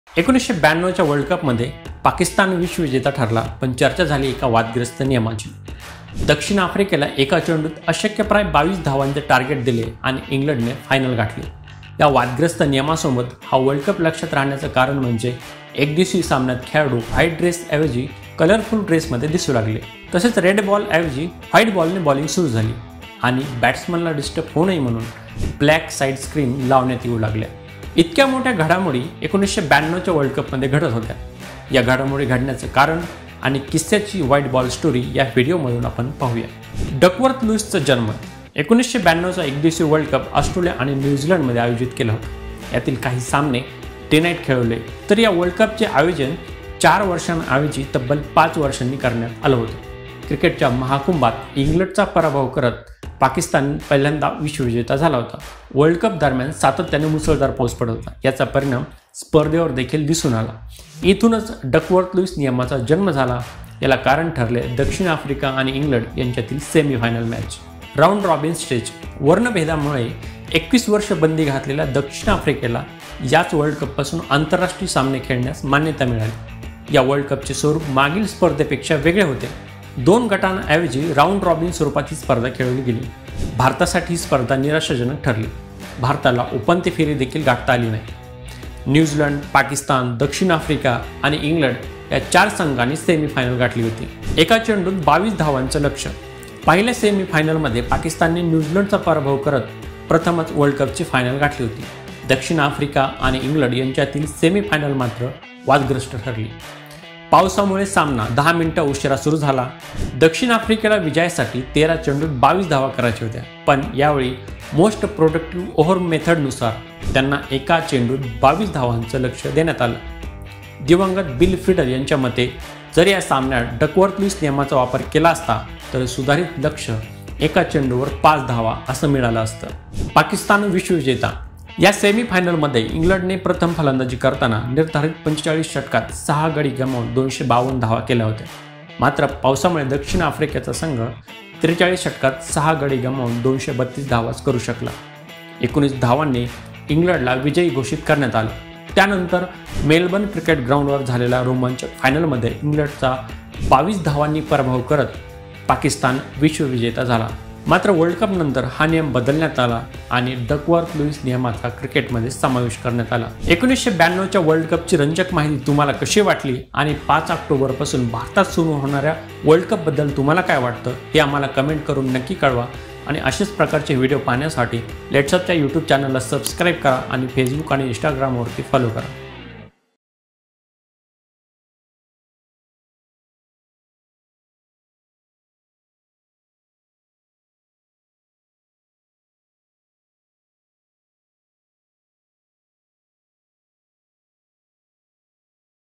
World Cup 1992 Memories | एक चेंडू 22 धावा अन् पाकिस्तान विश्वविजेता | LetsUpp Marathi
1992 च्या वर्ल्ड कपमध्ये पाकिस्तान विश्वविजेता ठरला पण चर्चा झाली ती एका वादग्रस्त नियमाची. वर्ल्डकपमधील वादाची कारणं आणि किस्से यांची वाईड बॉल स्टोरी..